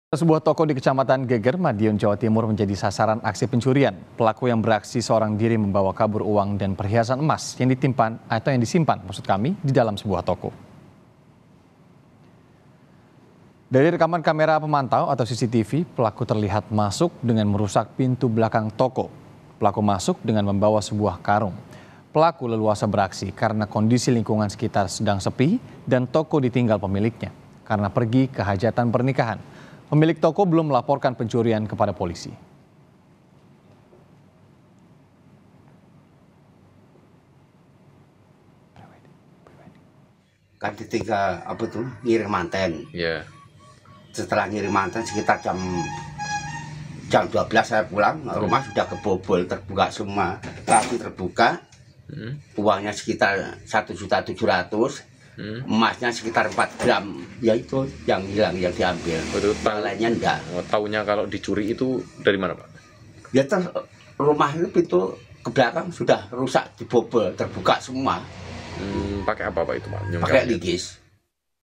Detik-detik Aksi Pencurian Sebuah Toko di Madiun
MADIUN, KOMPAS.TV - Sebuah toko di Kecamatan Geger, Madiun, Jawa Timur, menjadi sasaran aksi pencurian.

Pelaku yang beraksi seorang diri, membawa kabur uang dan perhiasan emas, yang disimpan di dalam toko.

Dari rekaman kamera pemantau, atau CCTV, pelaku terlihat masuk dengan merusak pintu belakang toko.

Pelaku masuk dengan membawa sebuah karung.

Pelaku leluasa beraksi, karena kondisi lingkungan sekitar sedang sepi, dan toko ditinggal pemiliknya, karena pergi ke hajatan pernikahan.

Pemilik toko belum melaporkan pencurian ini, kepada polisi.  (*)

Kompas TV
Independen | Terpercaya